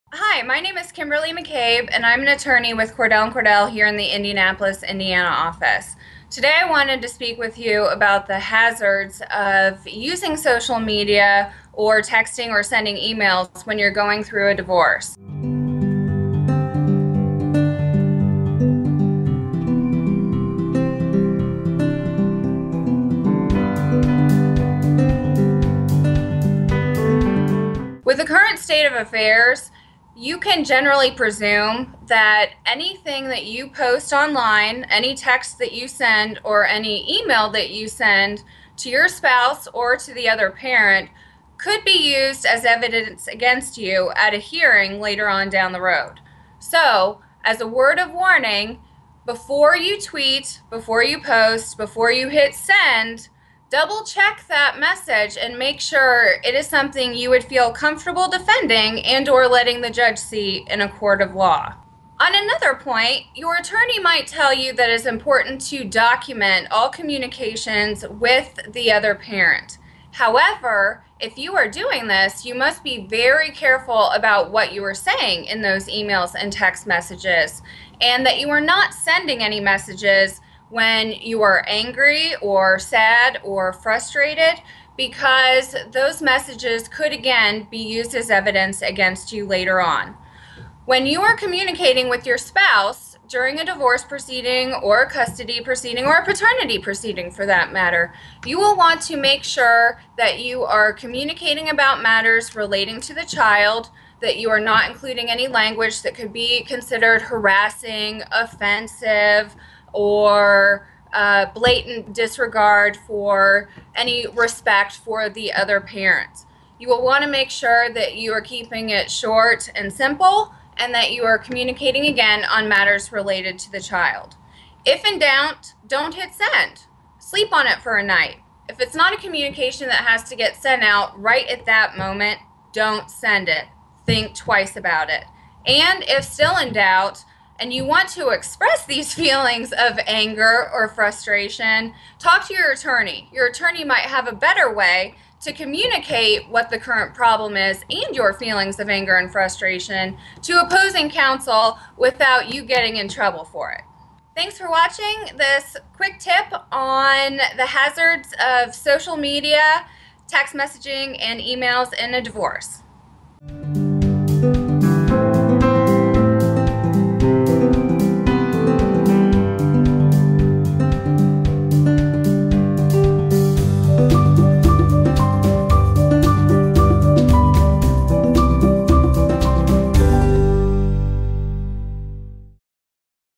The Hazards of Social Media, Texting and Email in Divorce - Attorney Bites
In this week’s video, Indiana family law attorney Kimberly McCabe explains how social media, texting and email can be dangerous during a divorce because of how easily it can be turned against you as evidence.

Set up a Consultation.